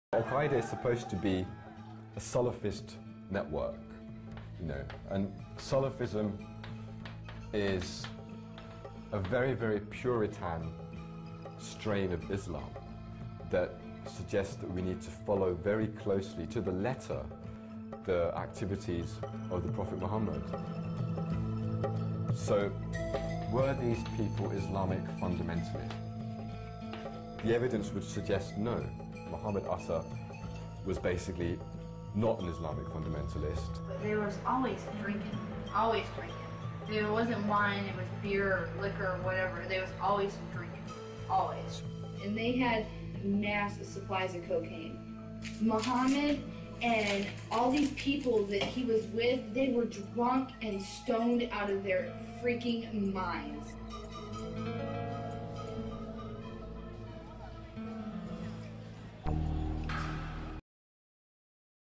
Mohammed Atta NOT an Islamist fundamentalist
Mohammed Atta [alleged Hijack ringleader] was NOT an Islamist fundamentalist. His former girlfriend Amanda Keller describes "he was always drunk and stoned on cocaine, ALL the TIME".